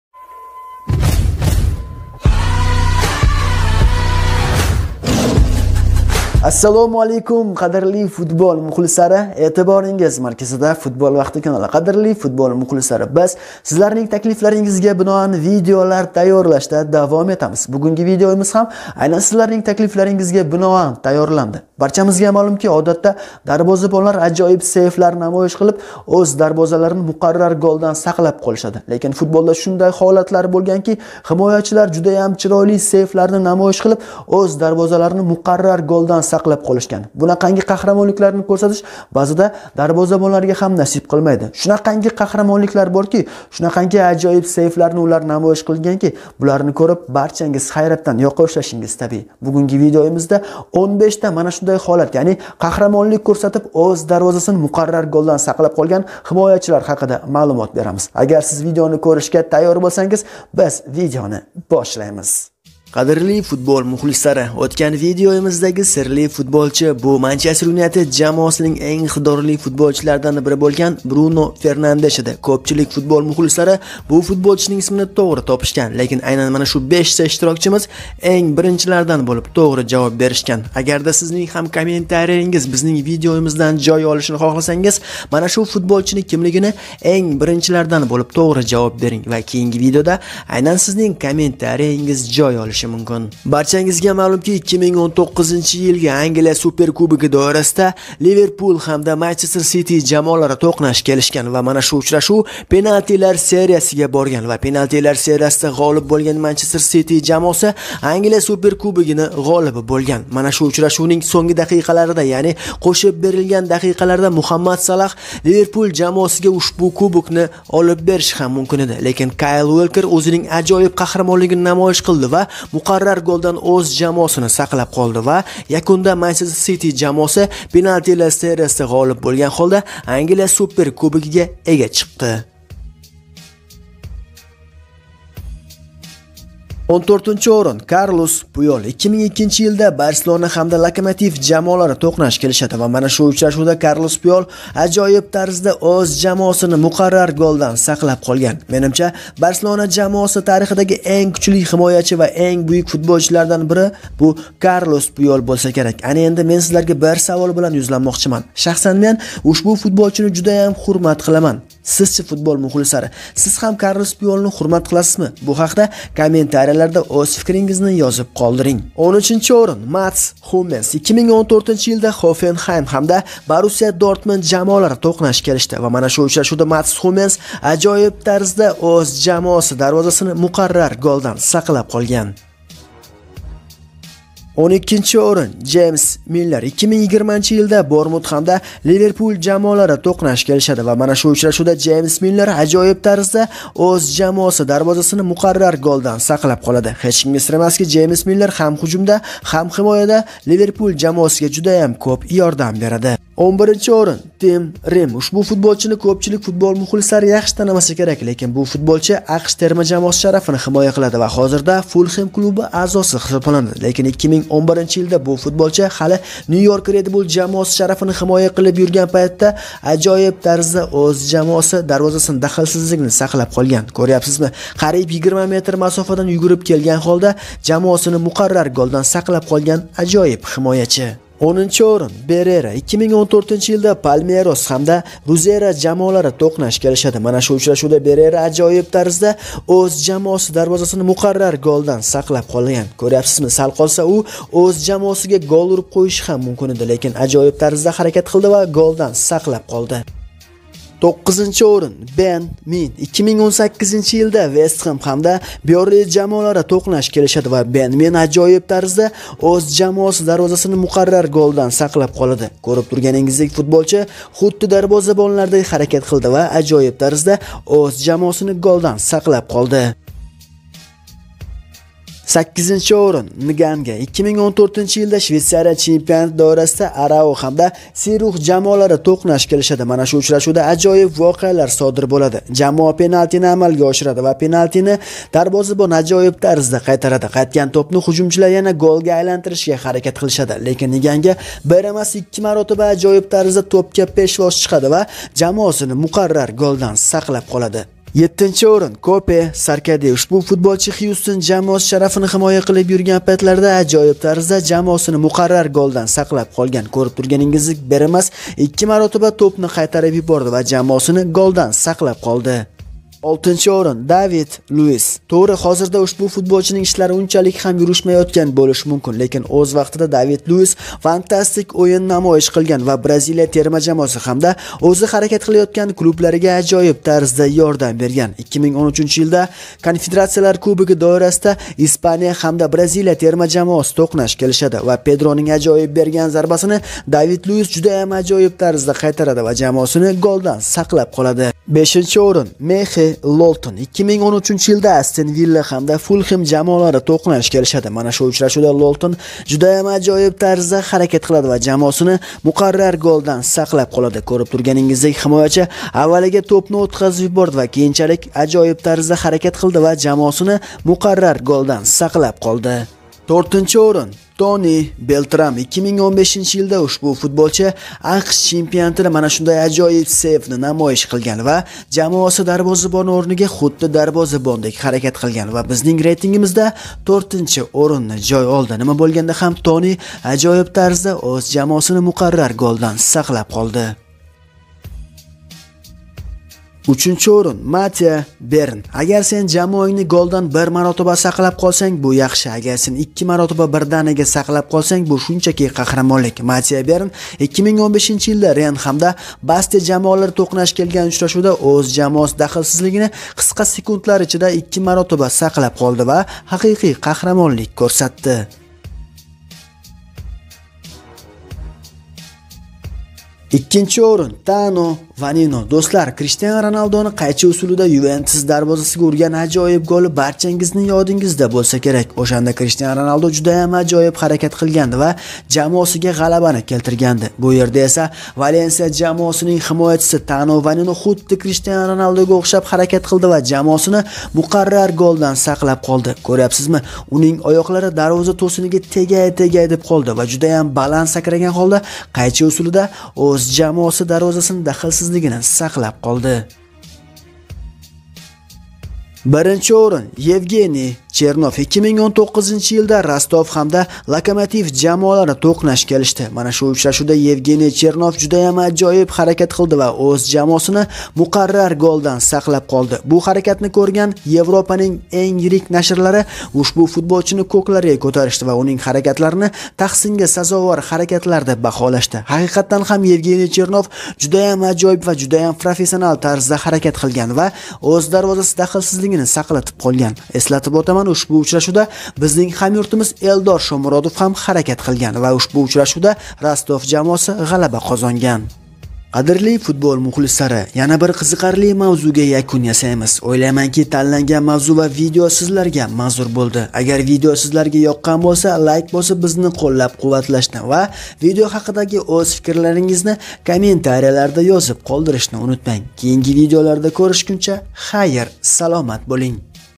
O'Z JAMOASINI MUQARRAR GOLDAN FANTASTIK TARZDA SAQLAB QOLGAN QAXRAMON FUTBOLCHILAR
ASSALOMU ALEYKUM BUGUN BIZ SIZLARGA O'Z JAMOASINI MUQARRAR GOLDAN FANTASTIK TARZDA SAQLAB QOLGAN QAXRAMON FUTBOLCHILAR HAQIDA VIDEONI HAVOLA QILAMIZ VIDEO HAQIDA FIKRIZGIZNI KOMENTARIYALARDA YOZIB QOLDIRING!!!